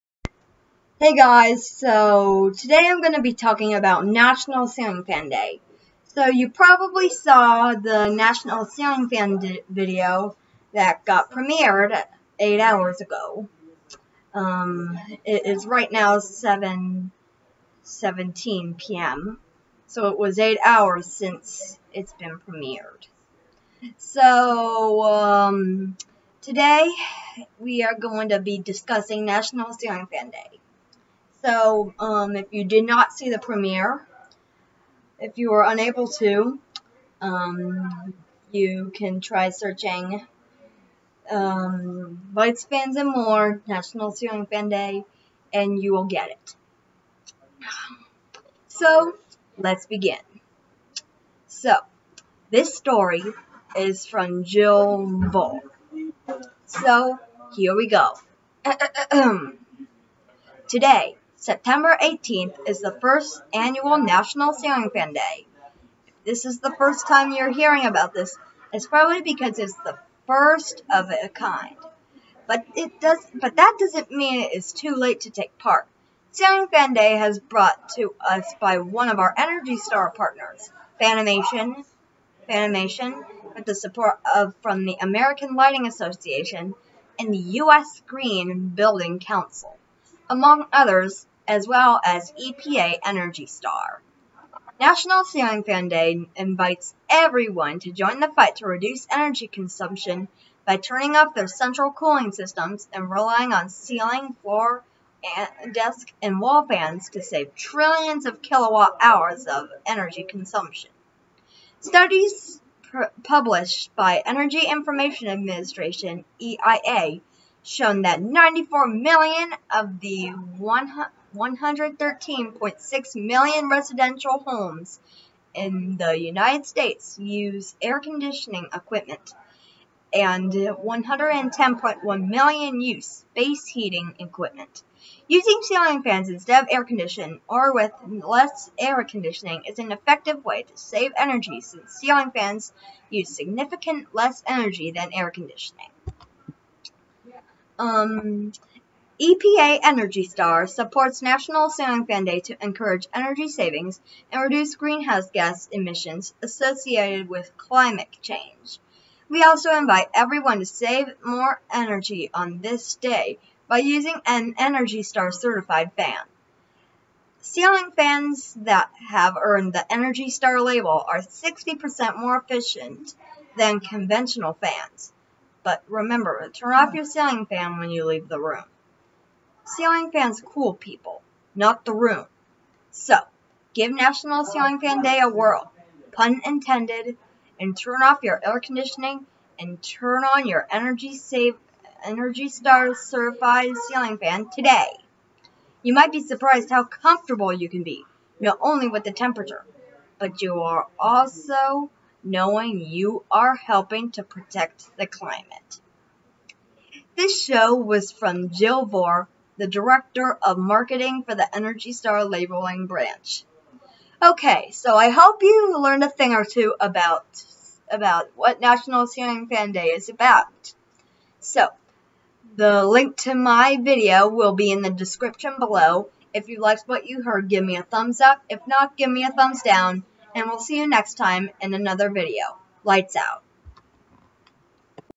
Lets talk about National Ceiling Fan Day
In this video we talk about National Ceiling Fan Day and how Ceiling Fans and Portable Fans conserve our energy bills. If you haven't watch our National Ceiling Fan Day video.